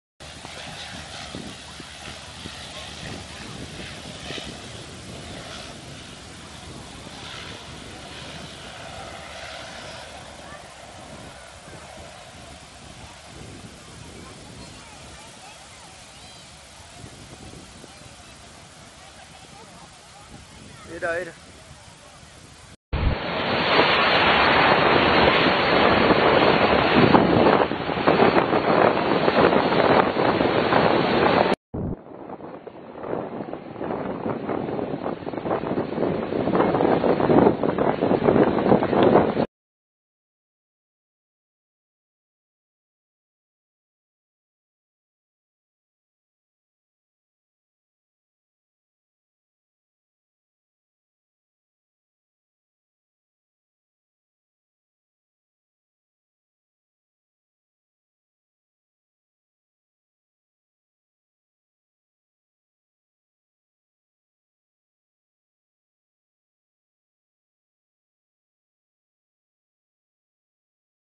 Hidroavión aterriza en la playa de Punta Umbría
La presencia de un hidroavión que sobrevolaba la orilla de la playa de Punta Umbría sin previo aviso y por causas desconocidas despertó incertidumbre y temor entre los bañistas que allí se encontraban. Más información